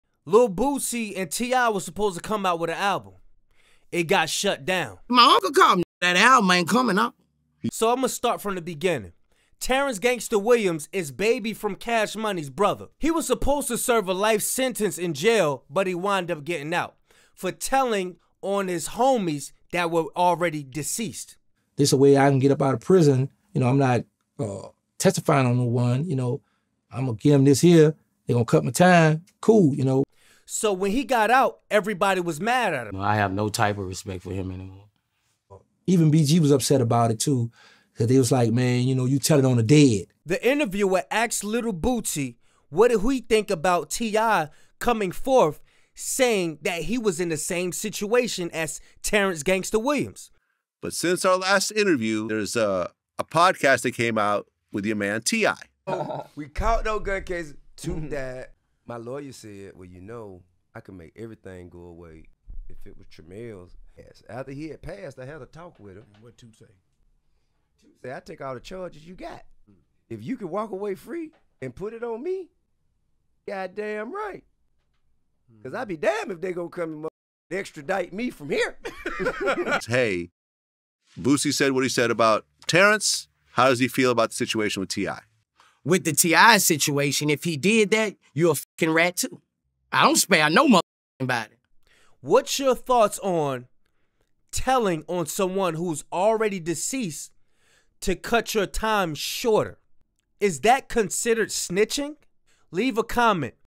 LIL BOOSIE CALLS OUT TI FOR SNITCHING!!CALLS OFF COLLABORATION￼ ALBUM
The album between Lil Bootsy and T.I. never came to be... But we have the story behind this unfortunate event